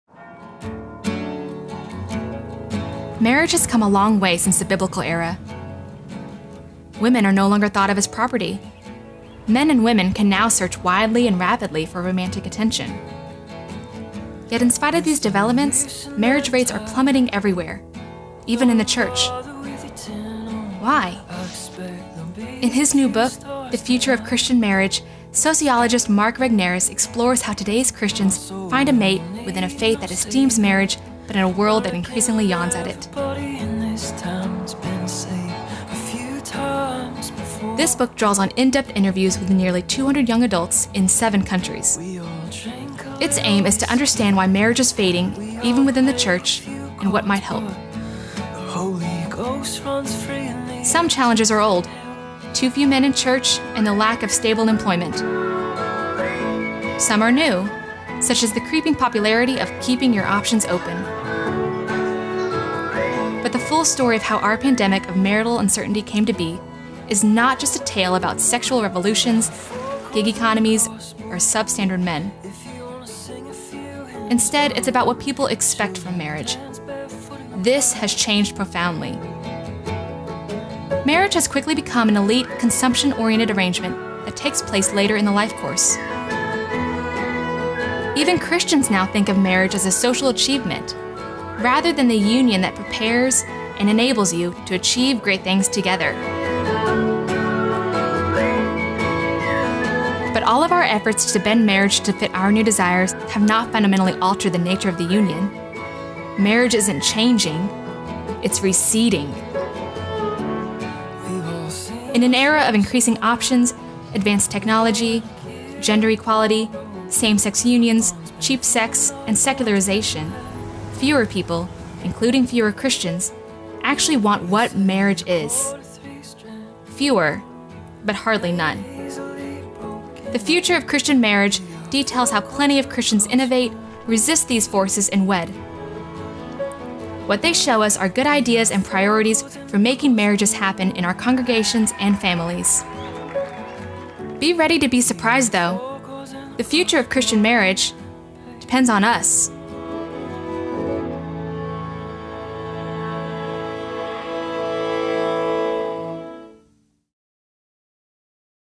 "The Future of Christian Marriage" By Dr. Mark Regnerus - Book Trailer
Dr. Mark Regnerus's book "Future of Christian Marriage" is available for purchase on Amazon

Marriage has come a long way since biblical times. Women are no longer property, and practices like polygamy have long been rejected. The world is wealthier, healthier, and more able to find and form relationships than ever. So why are Christian congregations doing more burying than marrying today? Explanations for the recession in marriage range from the mathematical--more women in church than men--to the economic, and from the availability of sex to progressive politics. But perhaps marriage hasn't really changed at all. Instead, there is simply less interest in marriage in an era marked by technology, gender equality, and secularization.

Mark Regnerus explores how today's Christians find a mate within a faith that esteems marriage but in a world that increasingly yawns at it. This book draws on in-depth interviews with nearly two hundred young-adult Christians from the United States, Mexico, Spain, Poland, Russia, Lebanon, and Nigeria, in order to understand the state of matrimony in global Christian circles today. Regnerus finds that marriage has become less of a foundation for a couple to build upon and more of a capstone. Meeting increasingly high expectations of marriage is difficult, though, in a free market whose logic reaches deep into the home today. The result is endemic uncertainty, slowing relationship maturation, and stalling marriage. But plenty of Christians innovate, resist, and wed, and this book argues that the future of marriage will be a religious one.